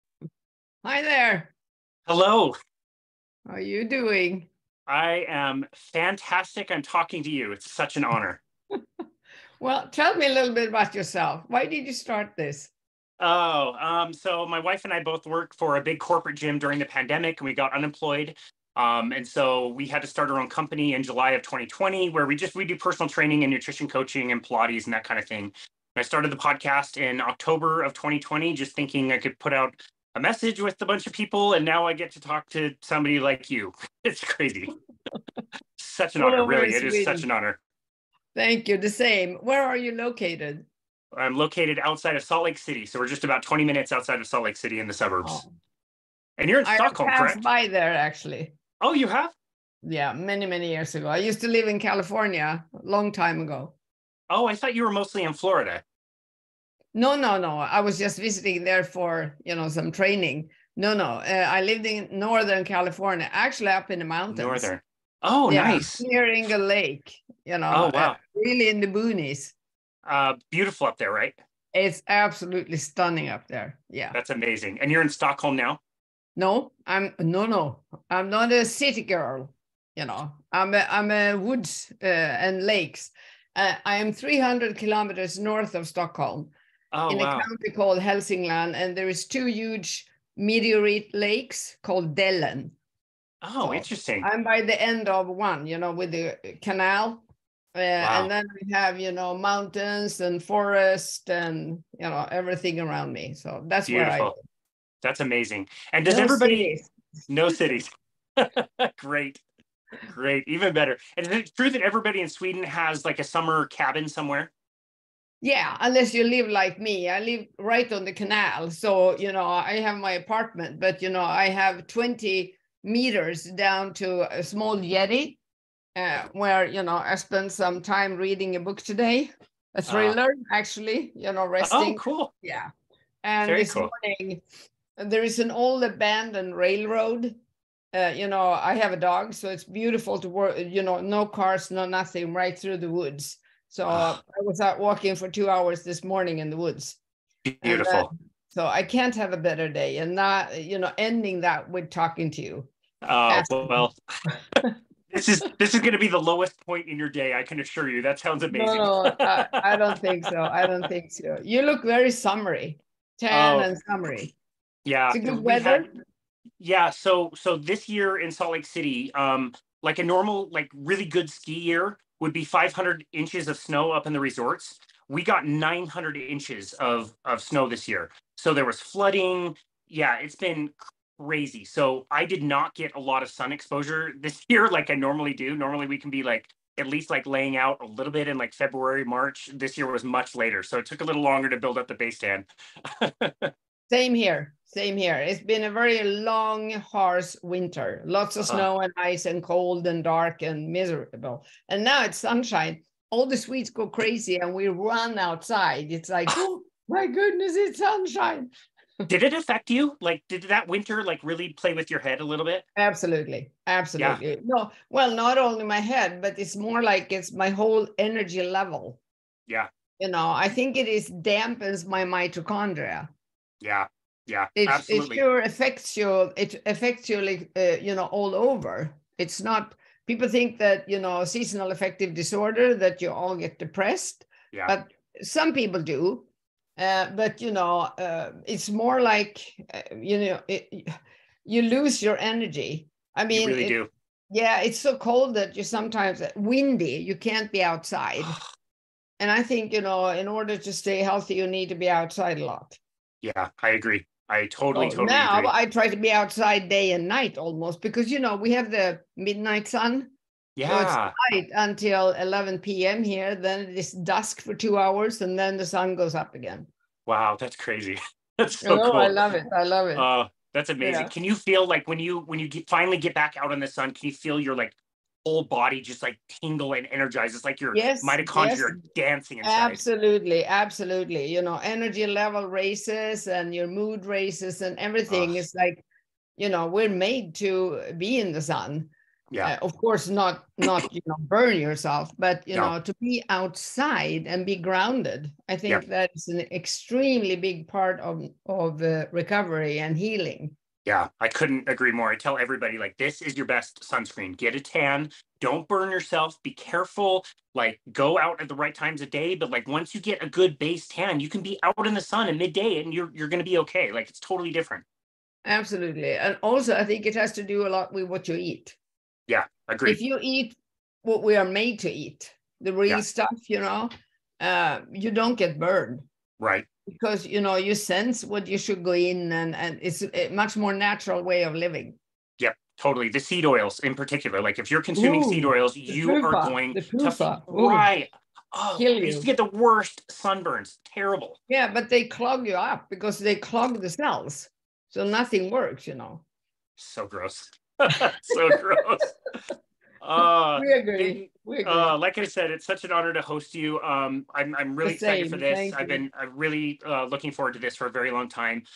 Boundless Body Candid- SUNSHINE EXPOSURE with World-Renowned Sugar Addiction Expert Bitten Jonnson!
A candid pre-interview chat with Bitten Jonsson, RN, is a US-trained expert on food addiction, who specializes in helping people to break free of unhealthy attachments to foods, such as sugar and processed carbohydrates.

Find Bitten at-

Find Boundless Body at-